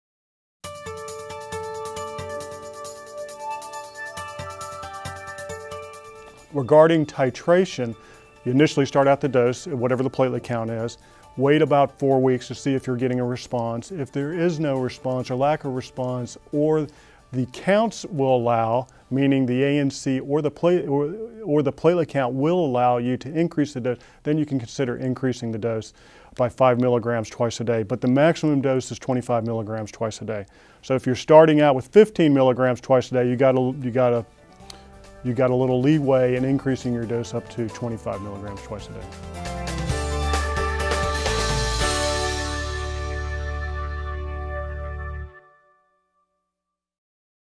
Initiating ruxolitinib therapy: Start with a high dose/titrate down, or to start lower/titrate up?
When initiating ruxolitinib therapy, is it better to start with a high dose and titrate down, or to start lower and titrate up?
Jeffrey Bryan, Pharm. D.
The University of Texas
MD Anderson Cancer Center